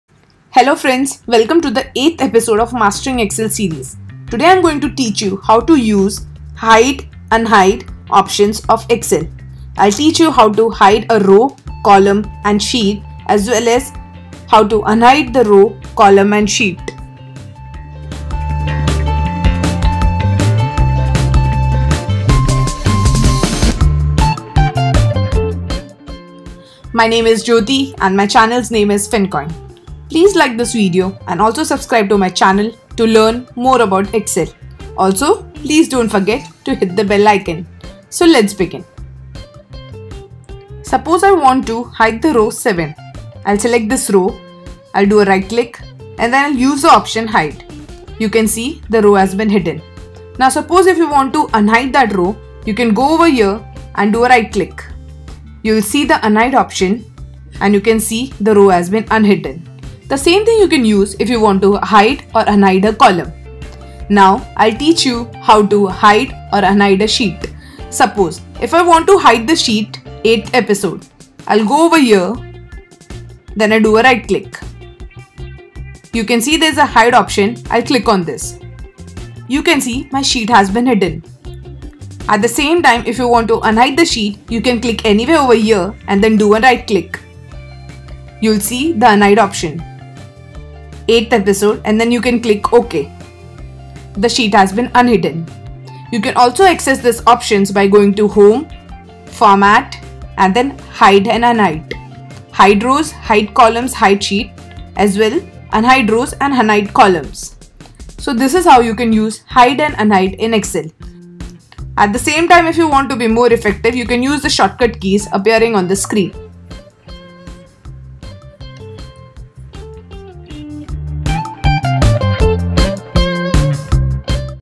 How to Hide Rows in Excel? | How to hide Sheets in Excel? | Shortcut keys | Windows | Mac | English
Learn how to Hide/Unhide Rows, Columns & Sheet in excel on both Mac and Windows operating system. Learn the shortcuts as well.